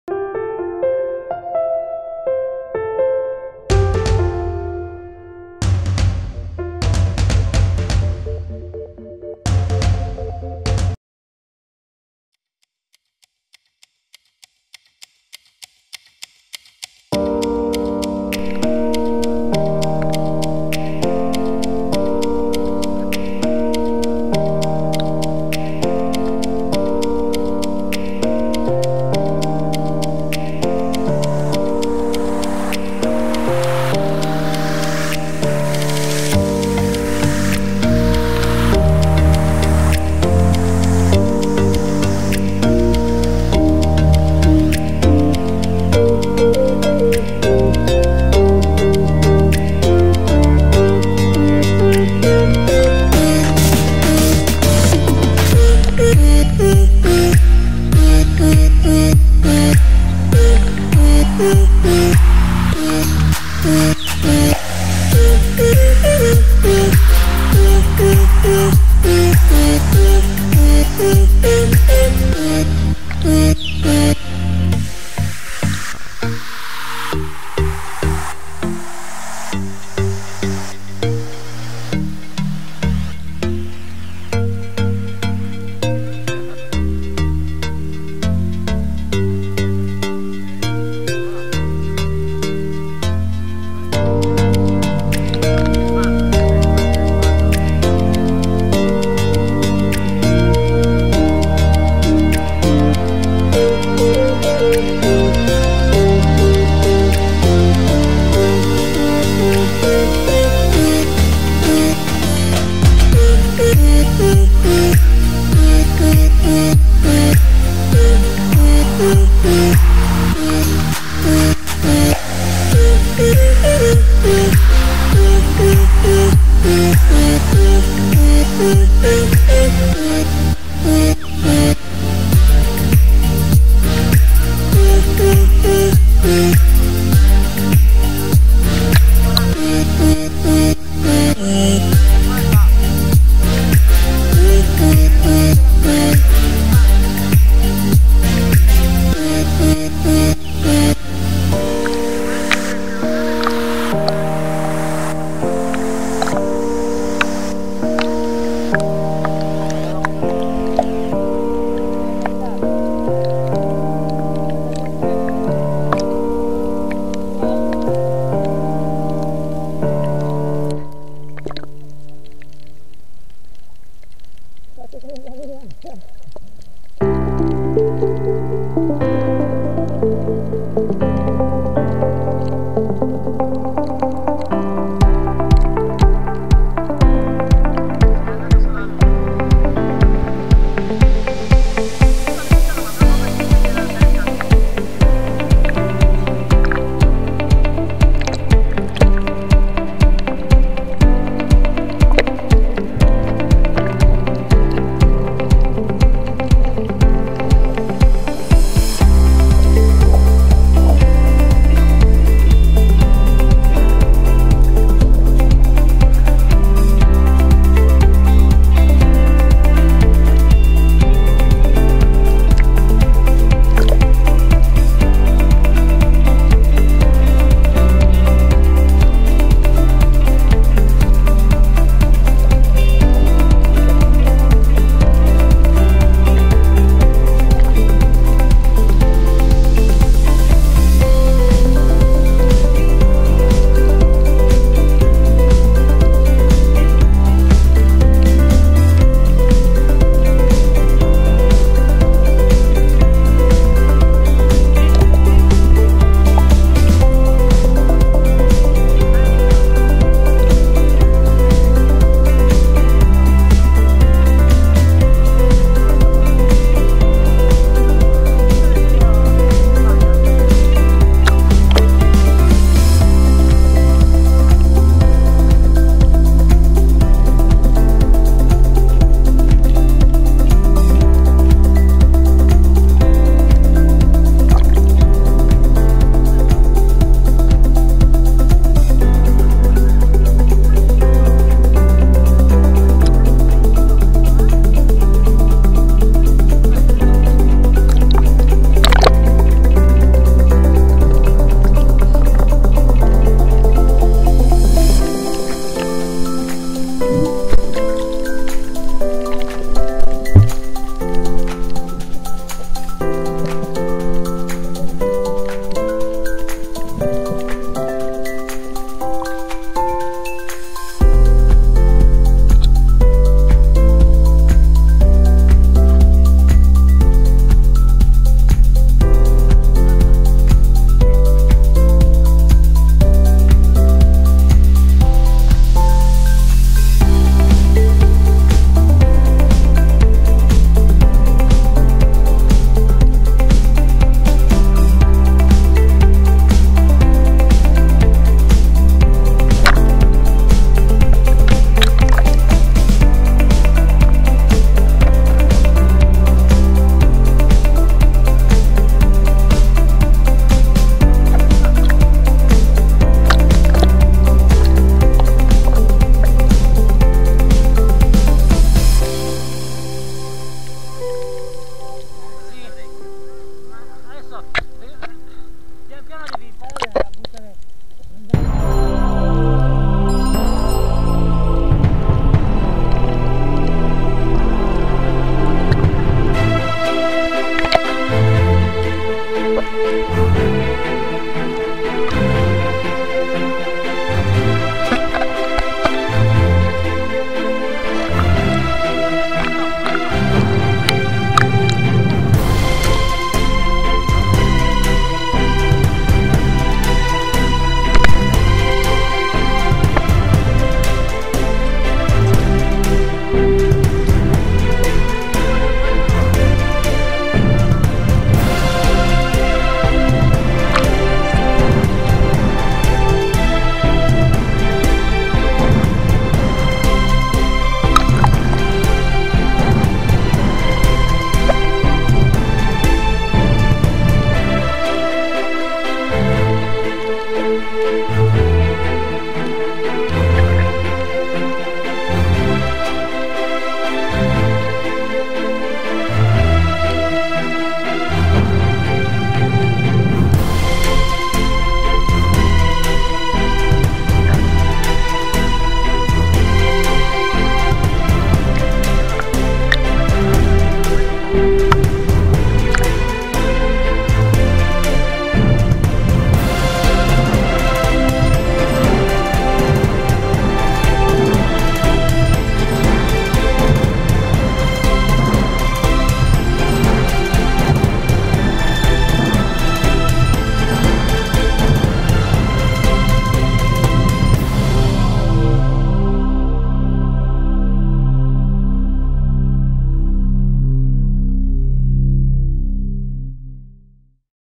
io & mio fratello Niko a isola delle femmine sicilia
stopcovid19 Io e mio fratello Niko a esplorare l'Isola delle femmine in sicilia